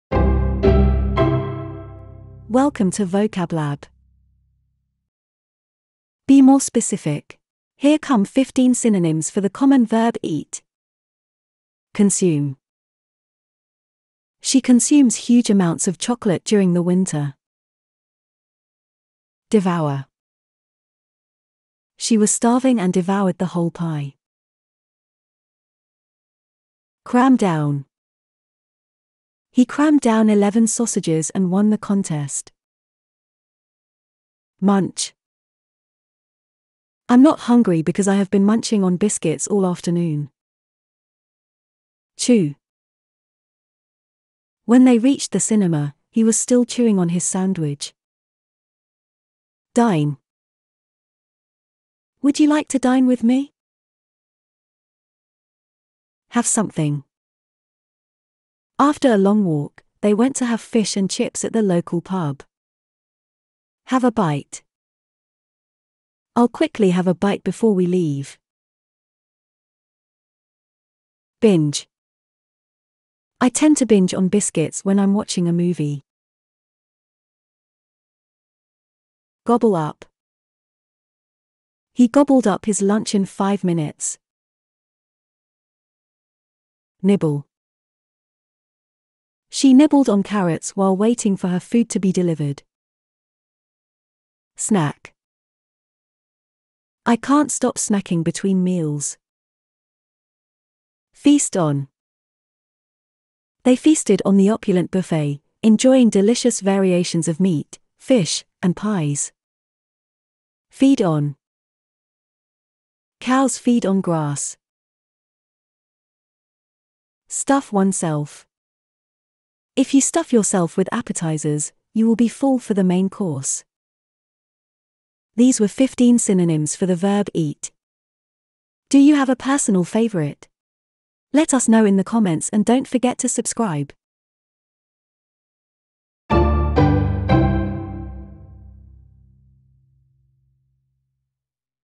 Improve your English skills! Learn 15 synonyms for the verb "EAT" - UK English vocabulary (advanced)
Welcome to Vocablab! The SYNONYMS series has been developed specifically for advanced English students. It aims to provide you with a broad and rich vocabulary for everyday and professional use.